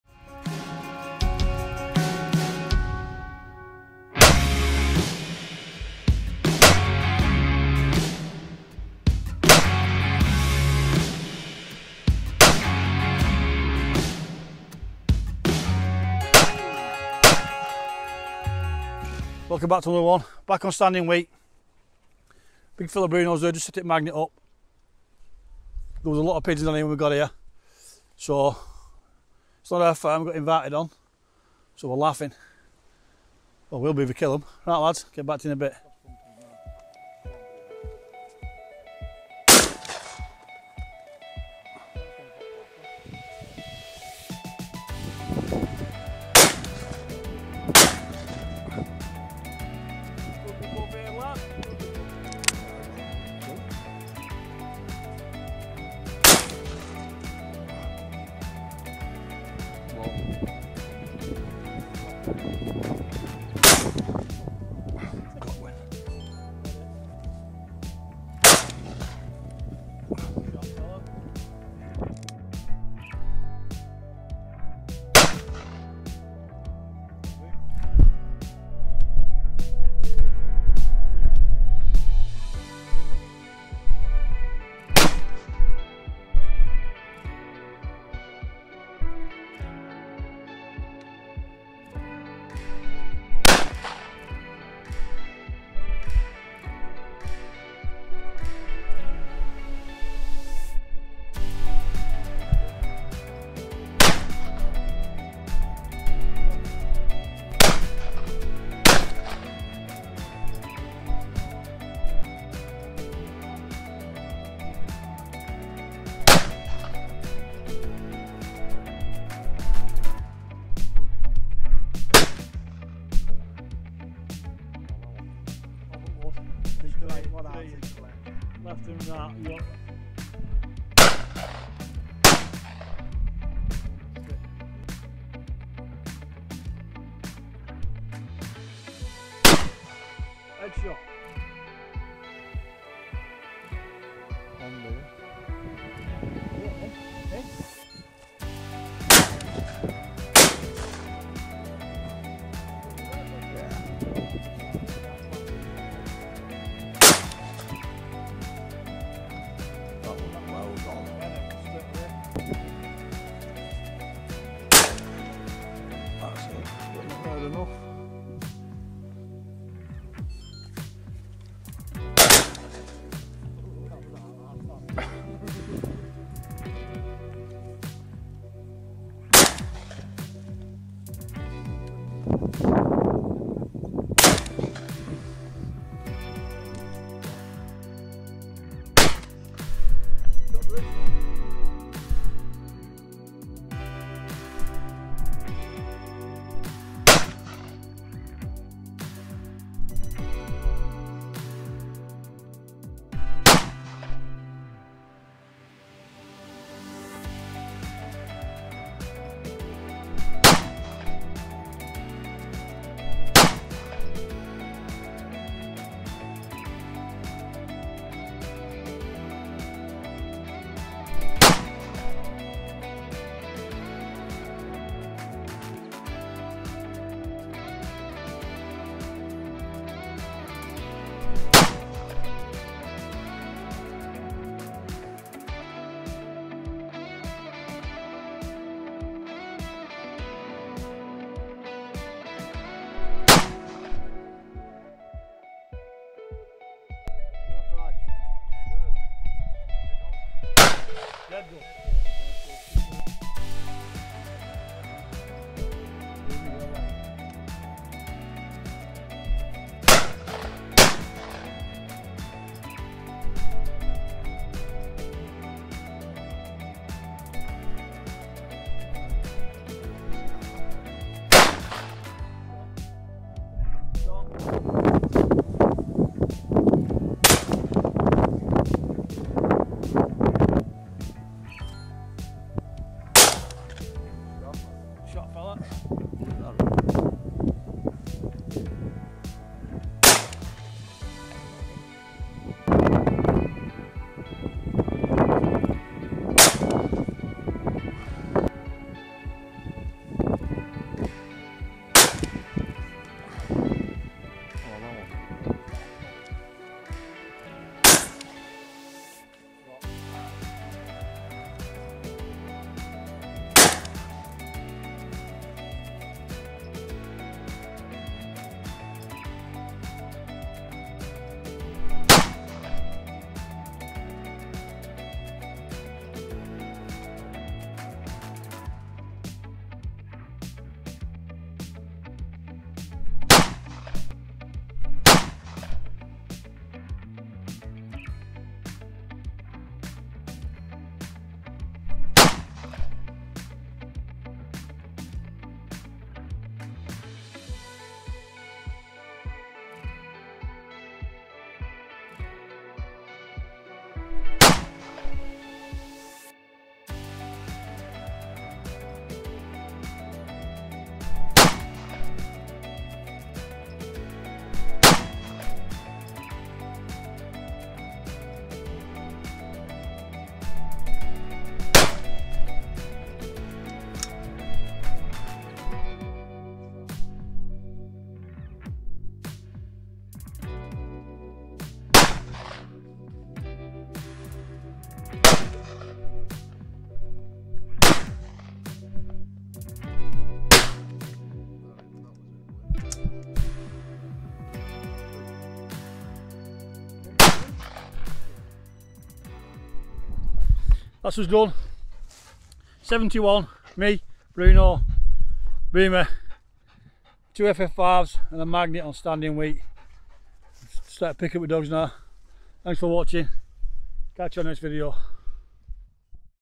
Pigeon Shooting !!! Wood Pigeon Shooting Using FF5 Flappers & A Pigeon Magnet
Pigeon Shooting !!! Wood Pigeon Shooting Using FF5 Flappers & A Pigeon Magnet, We are back out pigeon shooting on standing wheat using FF5  flappers & magnet, this combo works great for wood pigeon  shooting when you are doing crop protection in the early summer months.

Thank you for viewing our video's all the products that are used in our films can be found on our

Kind Regards DJ Decoys.com